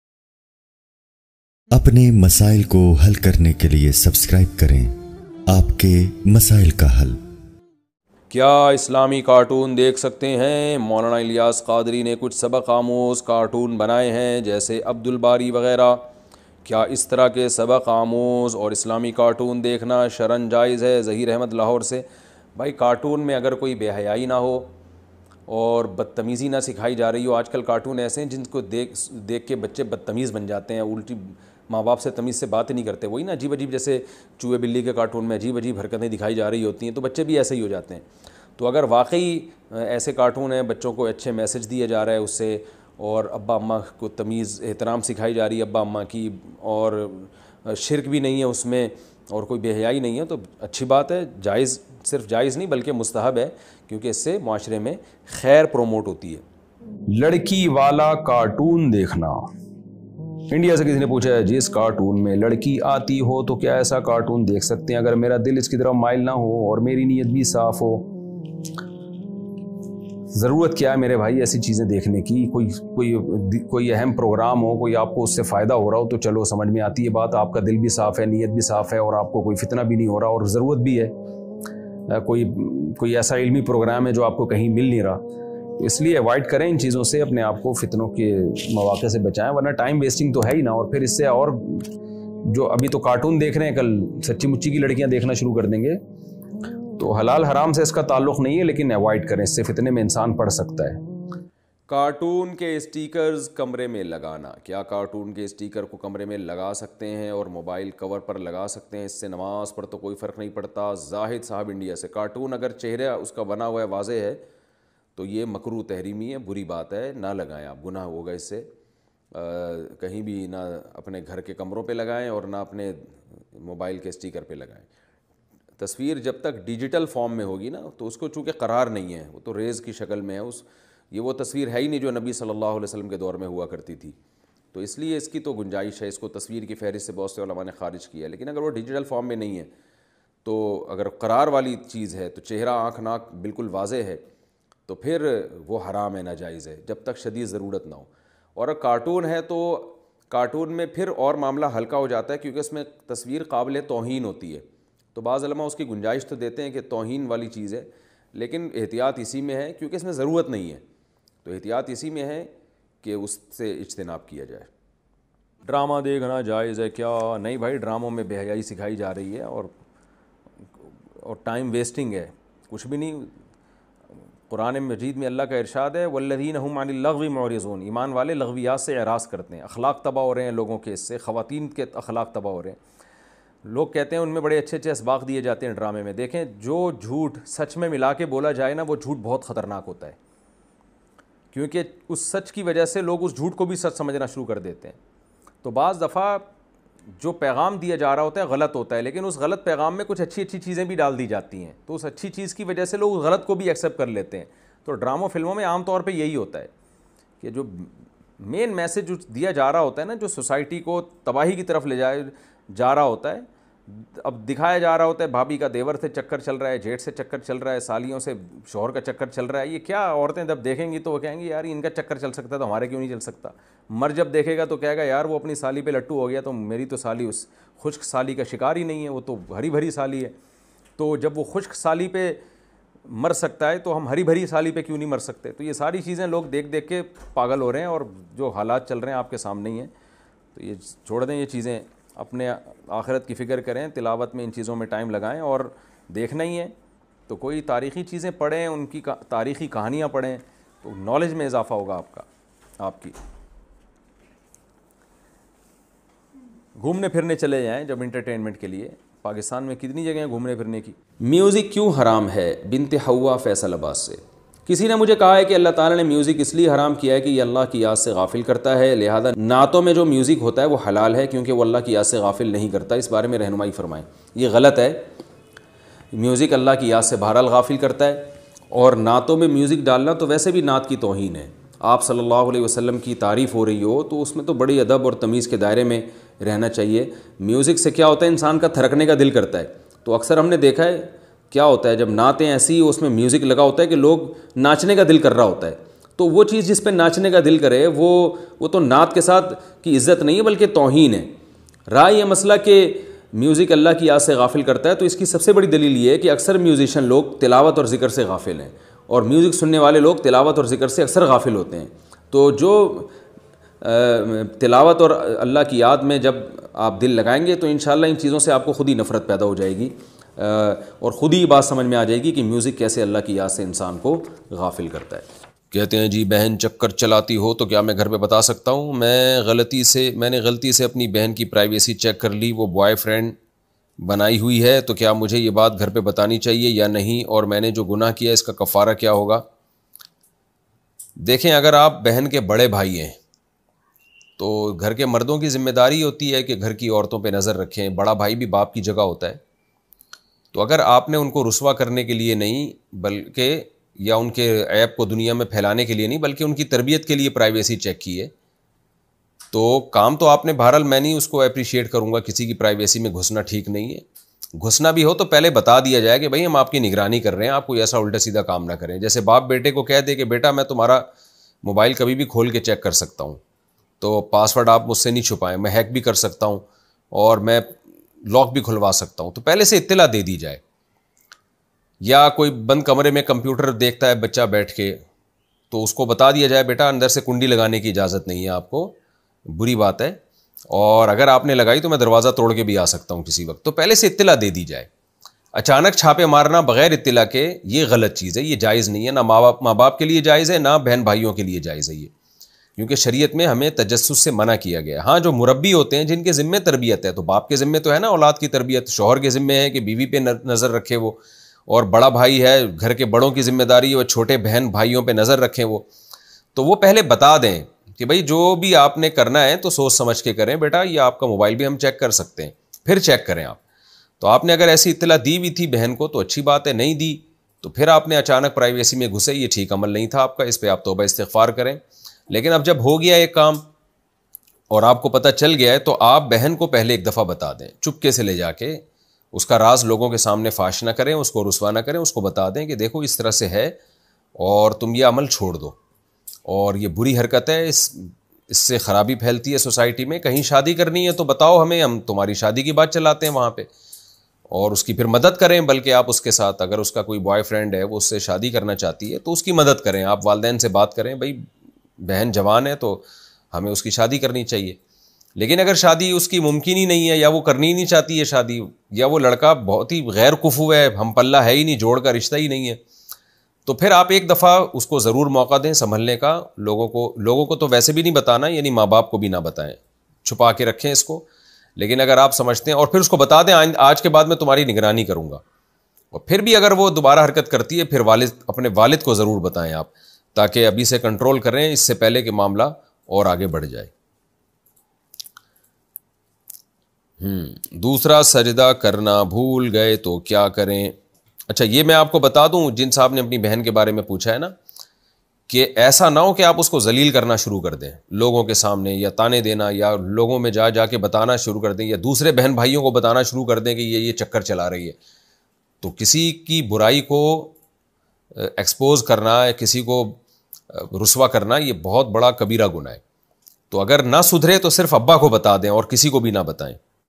Bachon Ko Cartoon Dikhane Wale Waldain Lazmi Sune | Mufti Tariq Masood | Aap Ke Masail Ka Hal
JazakAllah

जज़ाकल्लाह!